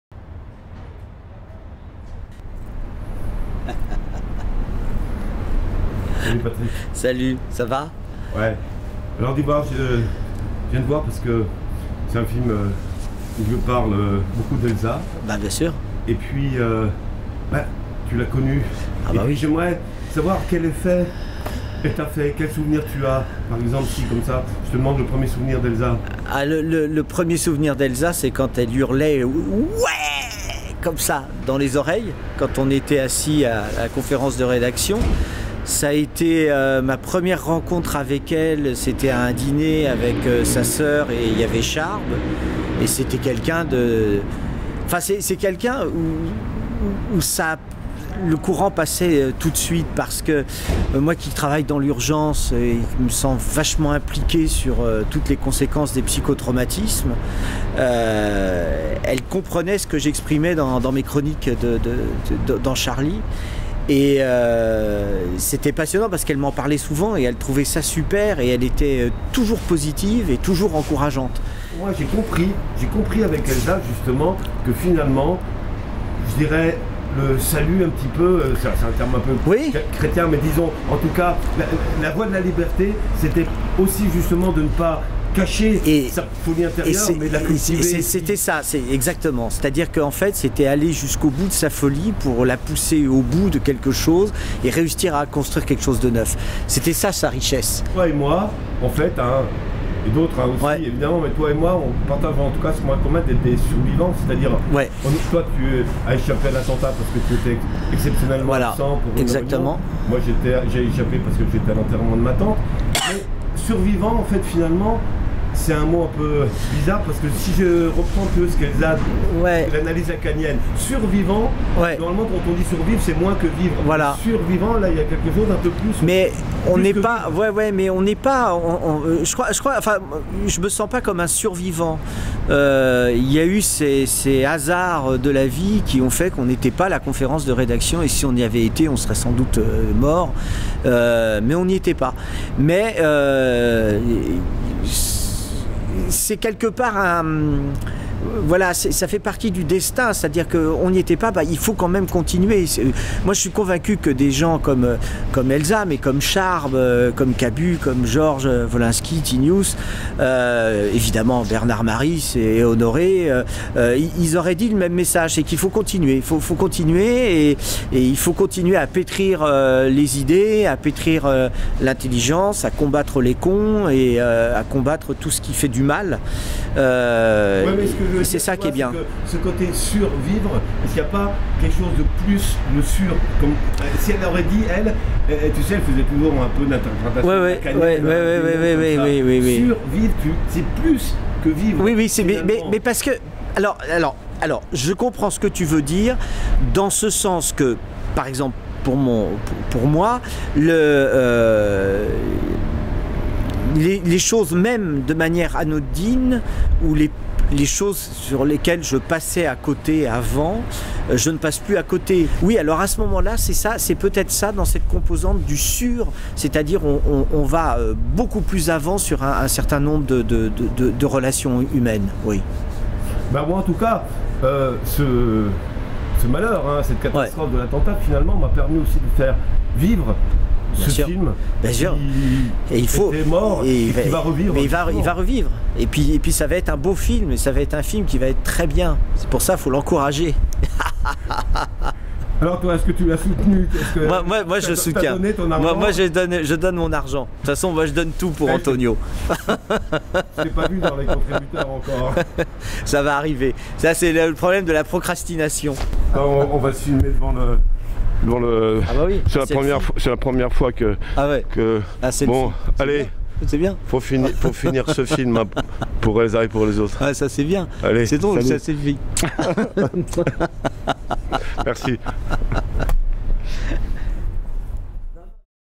"Toi et moi, nous sommes des sur-vivants"
Antonio Fischetti est journaliste à Charlie Hebdo. Il revisite les moments vécus avec la psychanalyste Elsa Cayat et la prostituée Momo, et les relie à son histoire avec Charlie depuis l'enfance jusqu'à aujourd'hui, pour tenter de comprendre le pouvoir des images sexuelles et religieuses. Une quête iconoclaste, intime et universelle.